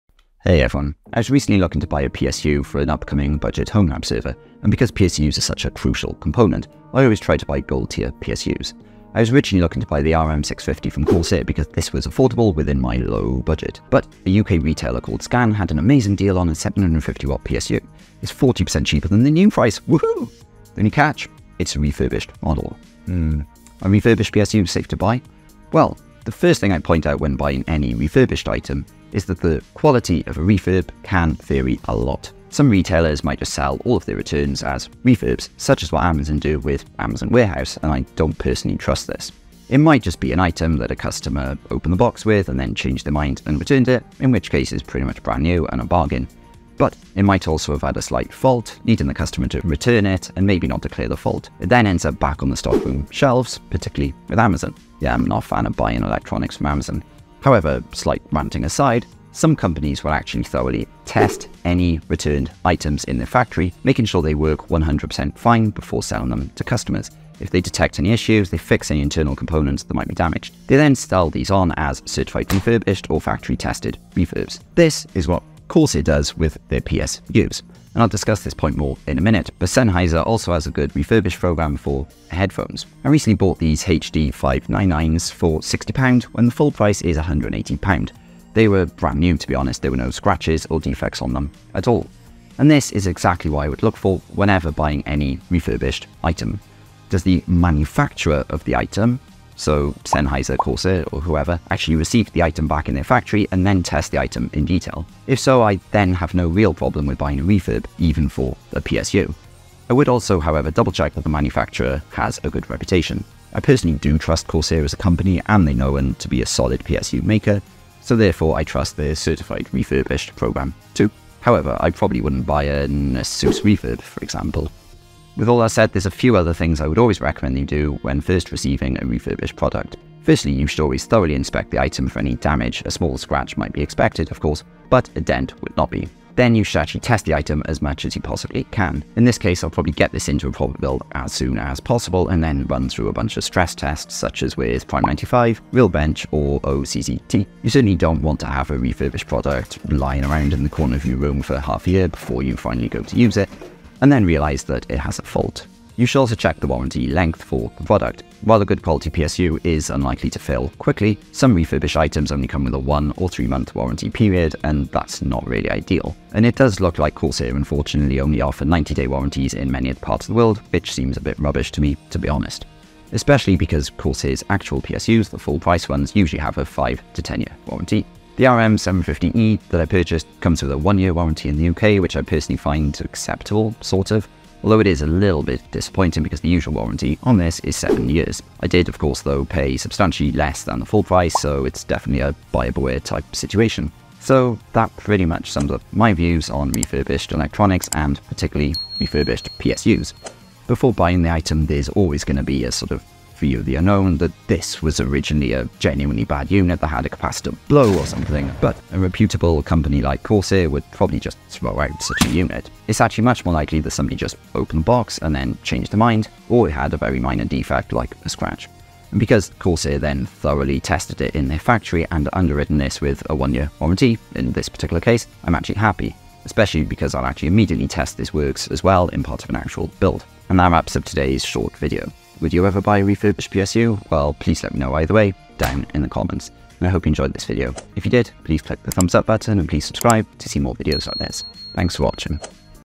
Is It Safe To Buy A Refurbished PSU?
I was recently looking to buy a budget 'gold tier' PSU to go in an upcoming HomeLab server build, and a deal flashed up: a Corsair PSU that usually costs £115, but was available for just £70. What gives? Why was it such a good deal? Well, because it was a refurbished PSU.

I wouldn't usually buy a refurb PSU, since it's such a crucial component, but I had a good experience with some Sennheiser refubished headphones so I thought "why not?".

In this video I also cover what you should check before (and after) buying a refurbished PSU, or even any refurb electronics.

00:00 Refurbished PSUs?
00:32 Bad Refurbs
00:56 Good Refurb Programs
02:05 Check This With A Refurbished PSU
03:15 It's Unlikely To Be A REALLY Faulty Item